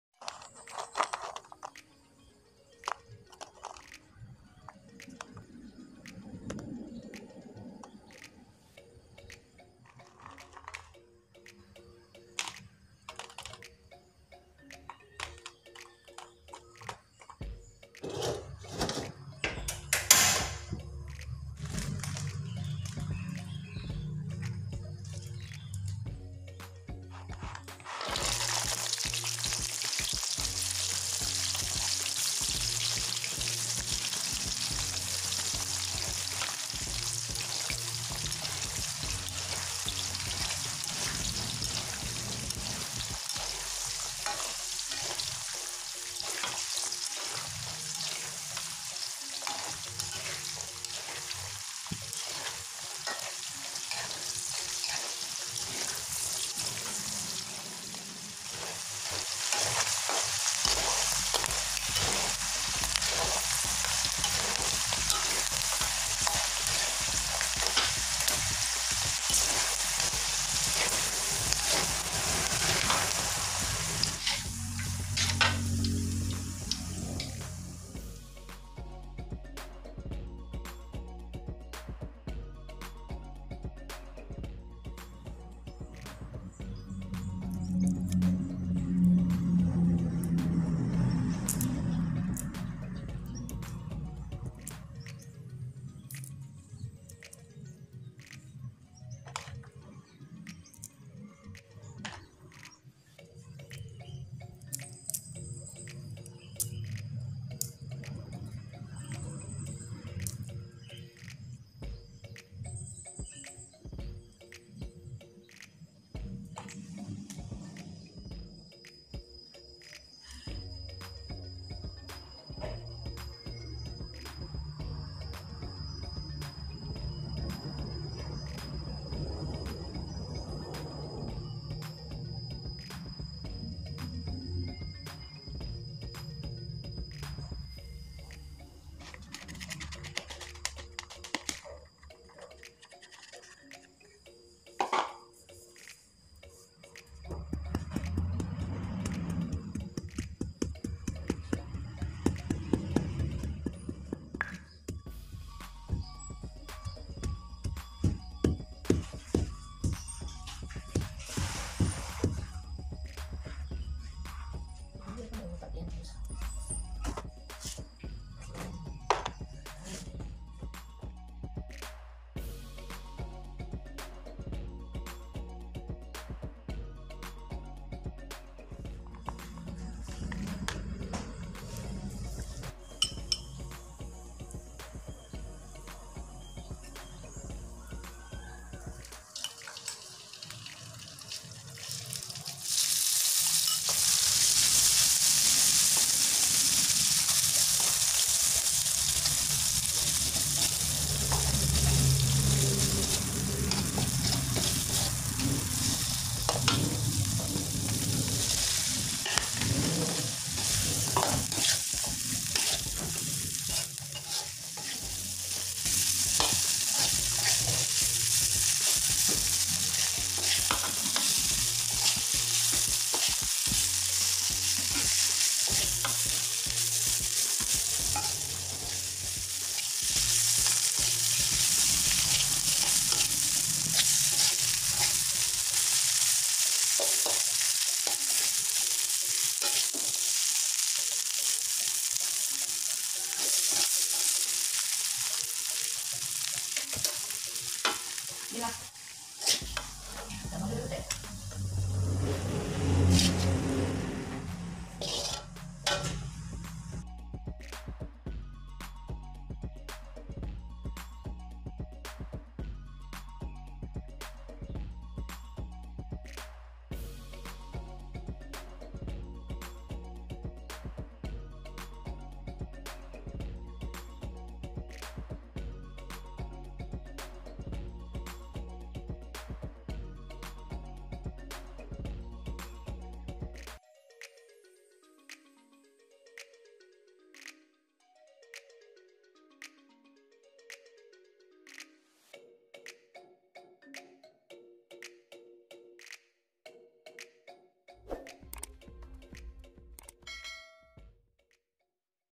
SAMBAL KABAU [EMI SL CHANNEL] #135
sambal kabau
bahan yang disediakan:
- kabau
- sambal tumis ( bawang merah, bawang putih, ebi, cabai, tomat dihaluskan) beri penyedap rasa, gula pasir dan garam